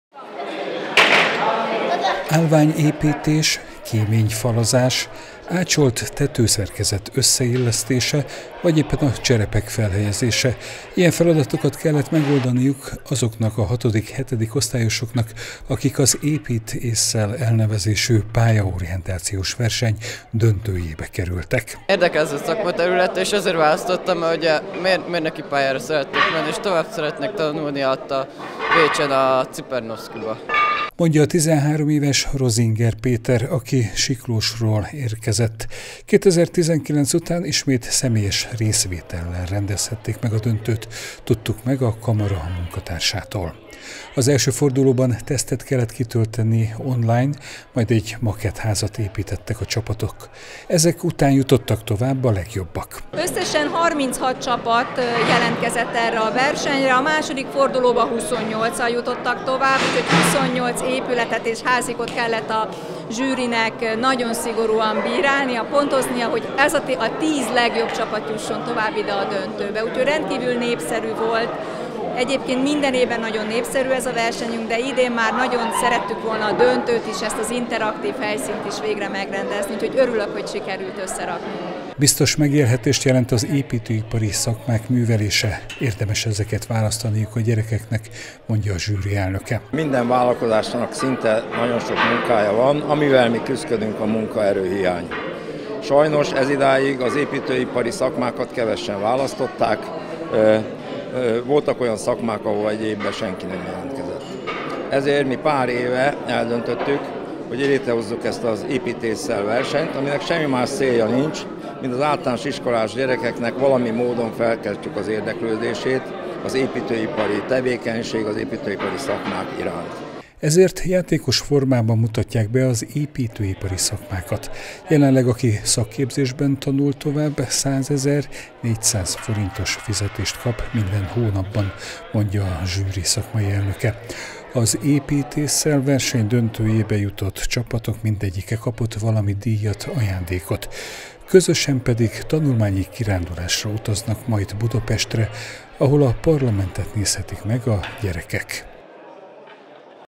Épít-Ésszel - szakmaismereti verseny
Milyen anyagokkal, szerszámokkal épülnek a házak? Hogyan kell a téglákat kötésbe tenni, vagy a cserepeket felrakni a tartólécekre? Ezekre a kérdésekre kaptak választ azok az általános iskolások, akik elindultak az Épít-Ésszel elnevezésű szakmaismereti versenyen. Itt játékos formában ismertették meg velük az építőipari szakmákat. Bízva abban, hogy a fiatalok egy-két év múlva ebben az irányban tanulnak tovább.